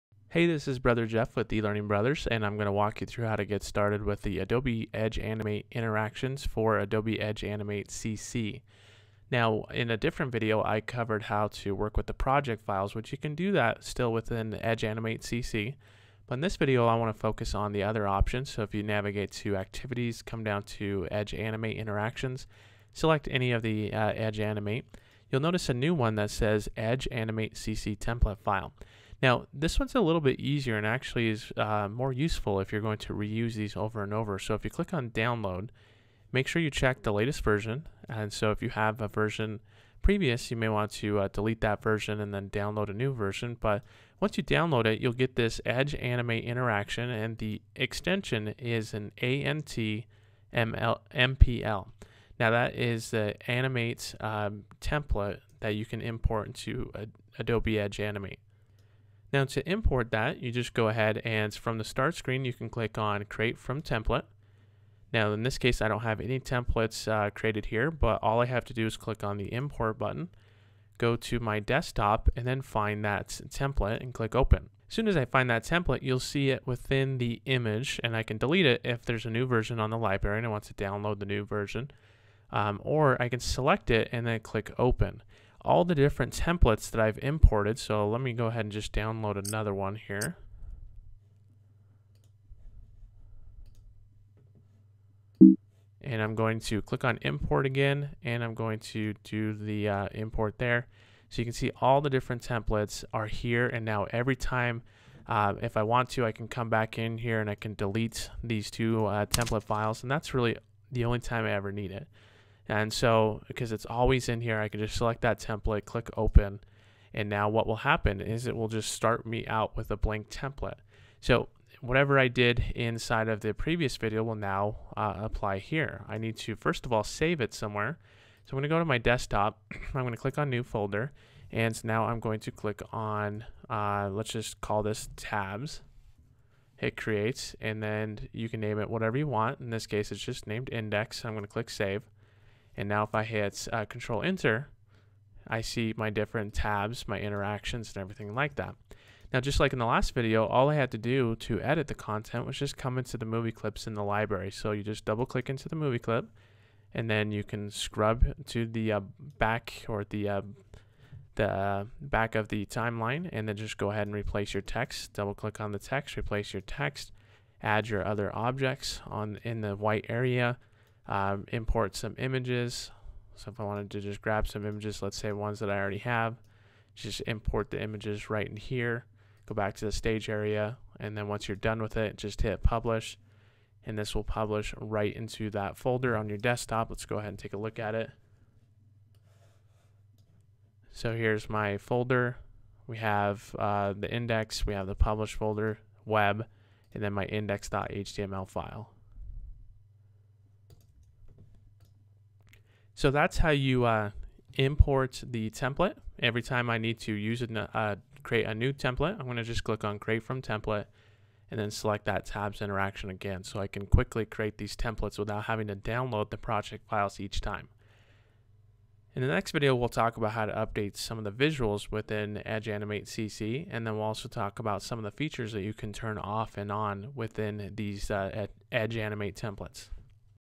Using the Edge Animate CC Templates from the eLB Library
With the introduction of Edge Animate CC, Adobe released a new way of handling templates. This allows you to install templates that you can quickly use over and over. In this video Brother Jeff will help you to get started with these templates.